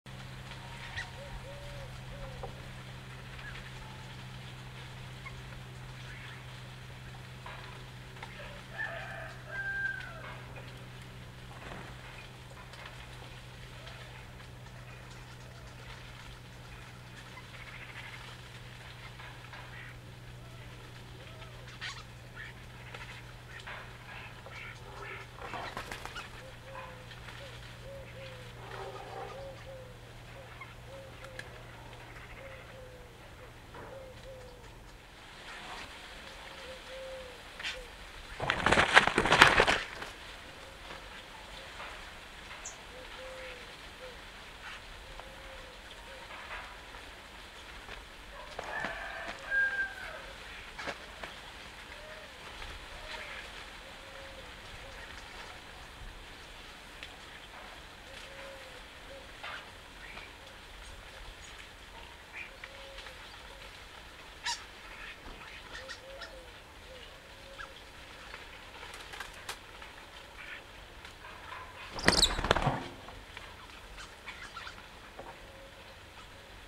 EPERVIER d'Europe/Attaque/Accipiter nisus ! BRUITX
Observation d'un couple d'Epervier d'Europe pendant l'hiver 2010/2011 près d'un dortoir d'Etourneaux sansonnet de plusieurs centaines d'individus chaque nuit !